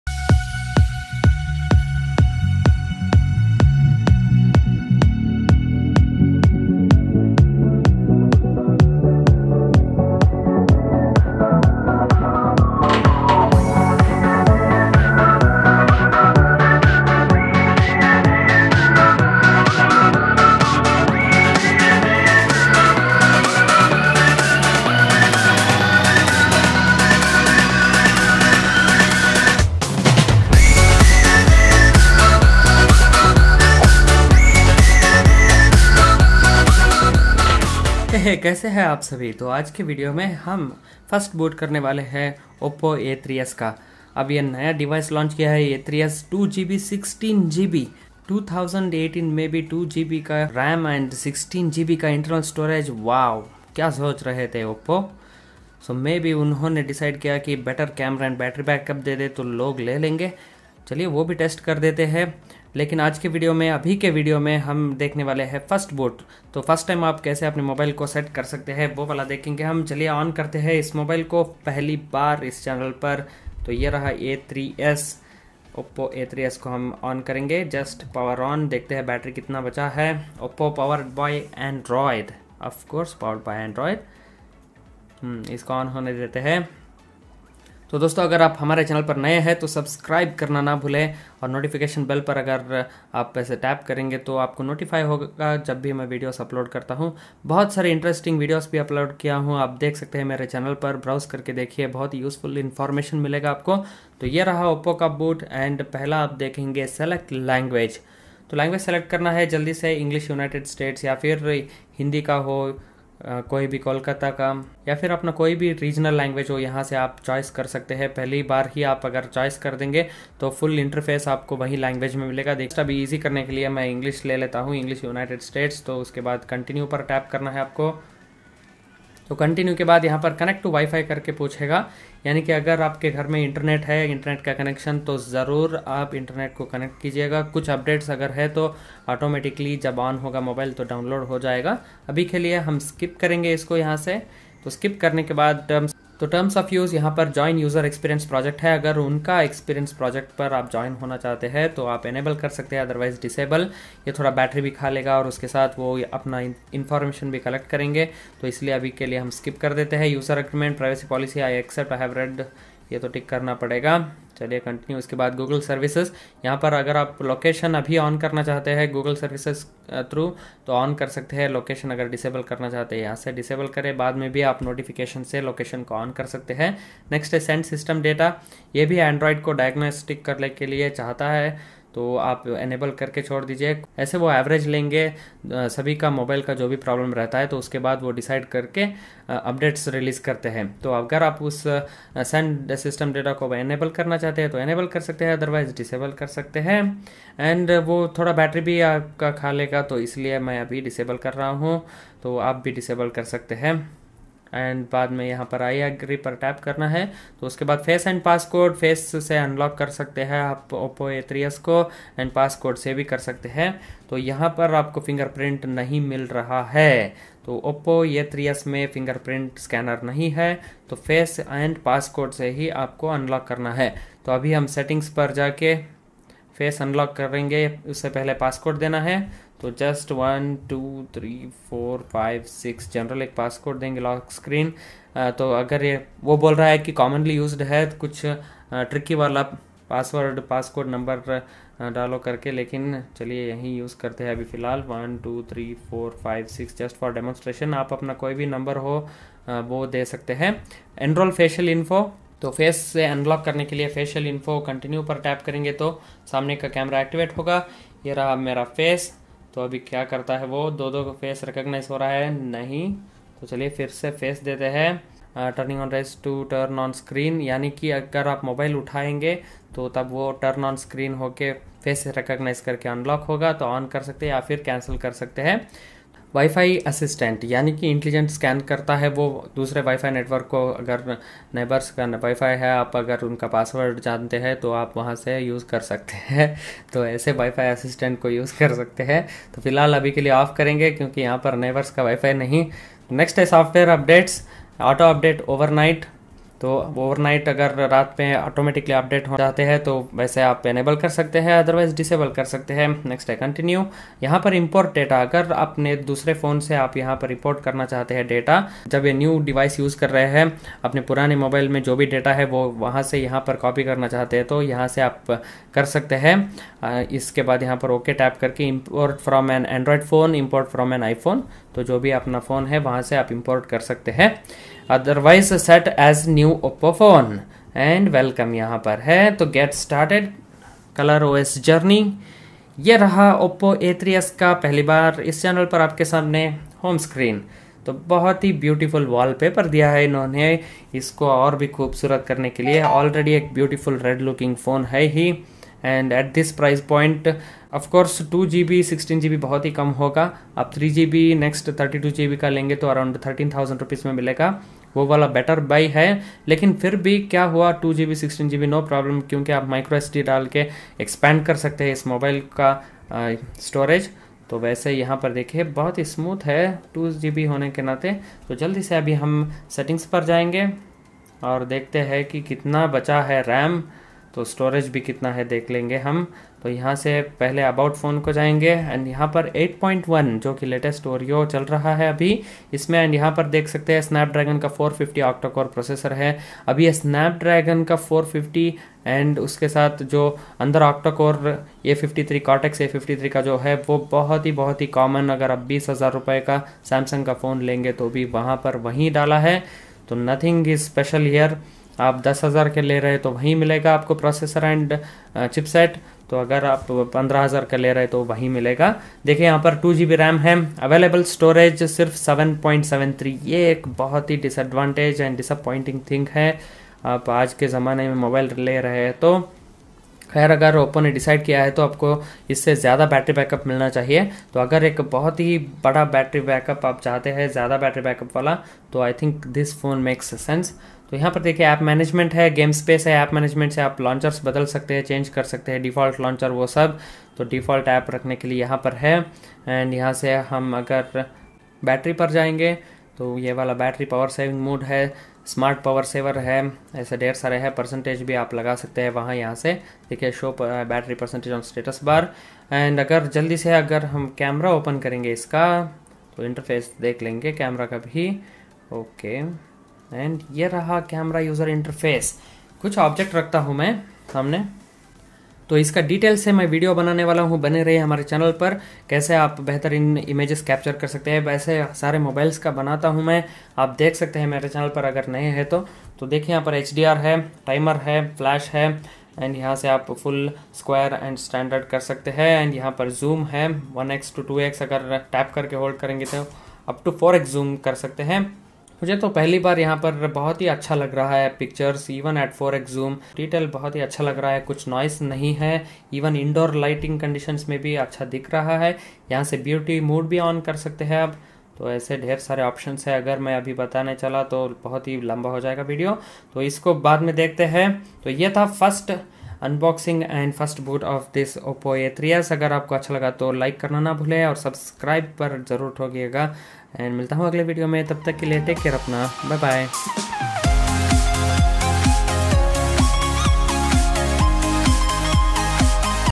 Oppo A3S Unboxing, First Time Setup & Overview in HINDI
In this Video, unboxing overview and first time setup of Oppo A3S.

इस विडियो में हम देखेंगे की kaise Oppo A3S ko first time boot kare aur naye phone ko kaise setup kare.

Oppo A3S quick Specs :
Display : 6.2" | 720 x 1520 pixels, 19:9 ratio (~271 ppi density)
Chipset: Snapdragon 450 + Cortex A53 CPU
Dual Rear Camera Setup : 13 MP, f/2.2, AF + 2 MP, f/2.4, depth sensor
Front Cam : 8MP f2.2
Weight : 168 g
Non-removable Li-Ion 4230 mAh battery

Oppo A3S Latest Updated Pricing